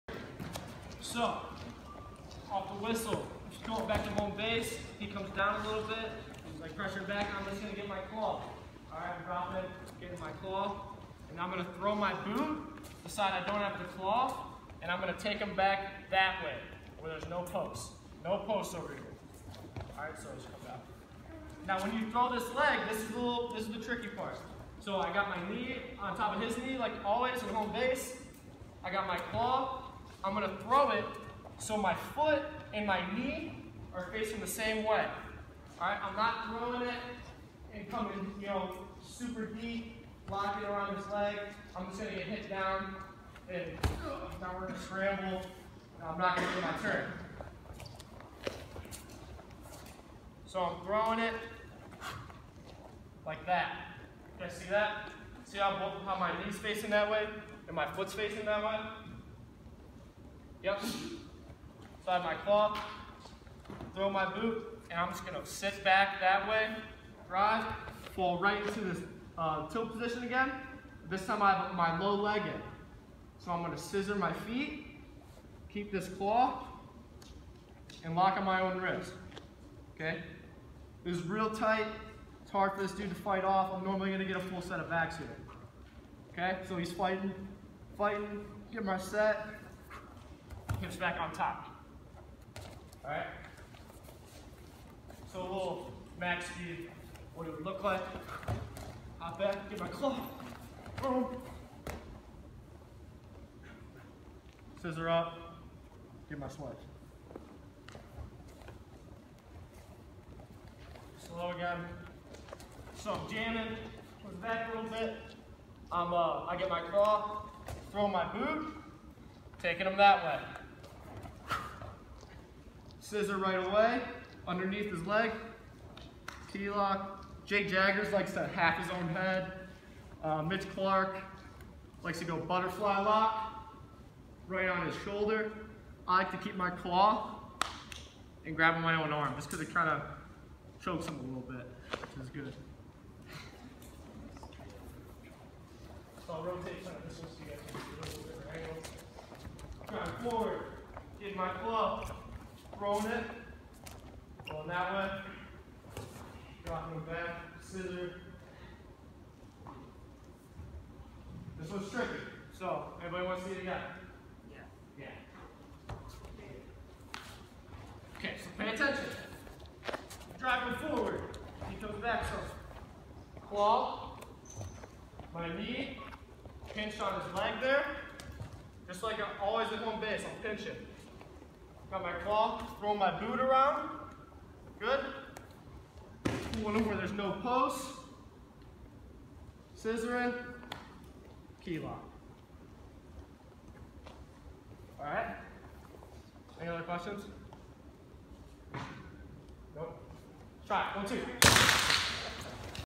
Ben Darmstadt Morning Session Camp 2018 #4
Ben Darmstadt demonstrates Claw & Farside Boot Tllt.